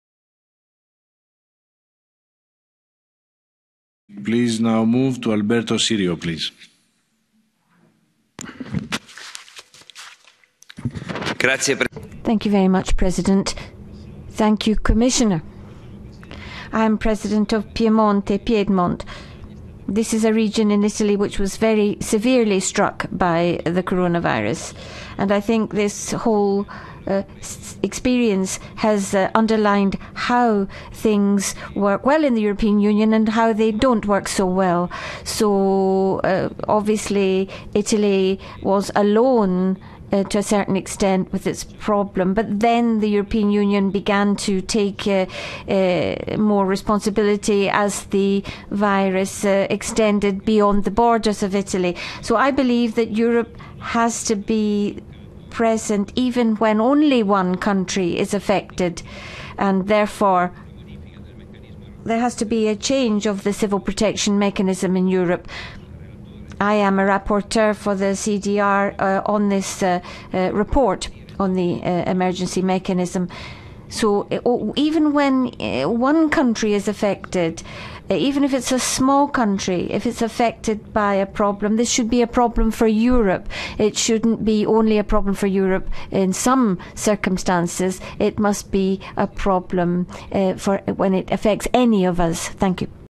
COVID-19 crisis management and the recovery plan - Alberto Cirio, President of the Piedmont Region
Alberto Cirio, President of the Piedmont Region at the 139th CoR Plenary Session talking about COVID-19 crisis management and the recovery plan